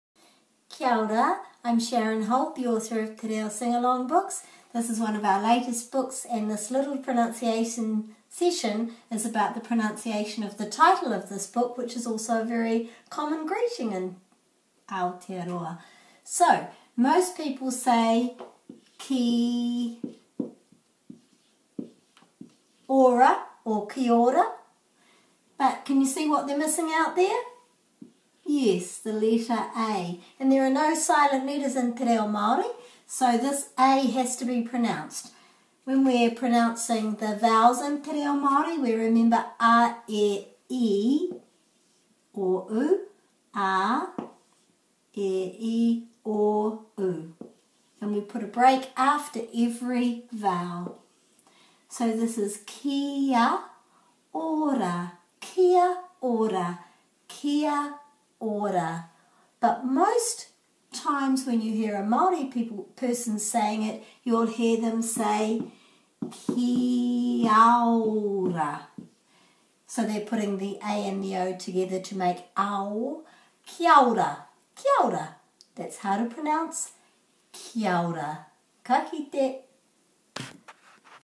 Kia ora - Te Reo Singalong book - Sharon Holt
Many people want to pronounce te reo correctly. This video from Sharon Holt, Te Reo Singalong books author, will help you to start that journey.